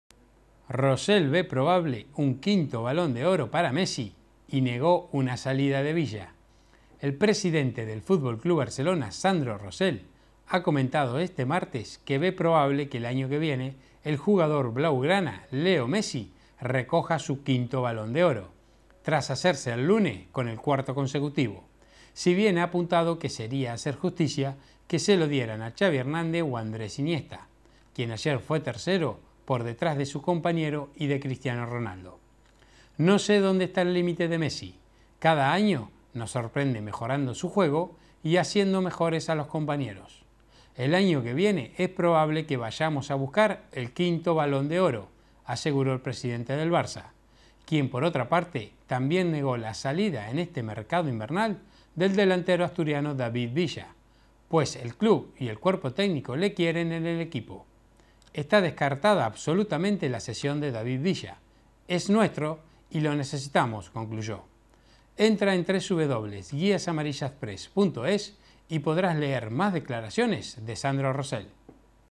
Rosell ve "probable" un quinto Balón de Oro para Messi y negó una salida de Villa
El presidente del FC Barcelona, Sandro Rosell, ha comentado este martes que ve "probable" que el año que viene el jugador blaugrana Leo Messi recoja su quinto Balón de Oro, tras hacerlo el lunes con el cuarto consecutivo, si bien ha apuntado que sería "hacer justicia" que se lo dieran a Xavi Hernández o Andrés Iniesta, quien ayer fue tercero por detrás de su compañero y de Cristiano Ronaldo.
"No sé dónde está el límite de Messi. Cada año nos sorprende mejorando su juego y haciendo mejores a los compañeros. El año que viene es probable que vayamos a buscar el quinto Balón de Oro", aseguró el Presidente del Barça, quien por otra parte también negó la salida en este mercado invernal del delantero asturiano David Villa, pues el club y el cuerpo técnico le quieren en el equipo. "Está descartada absolutamente la cesión de David Villa. Es nuestro y le necesitamos"concluyó. y podrás leer más declaraciones de Sandro Rosell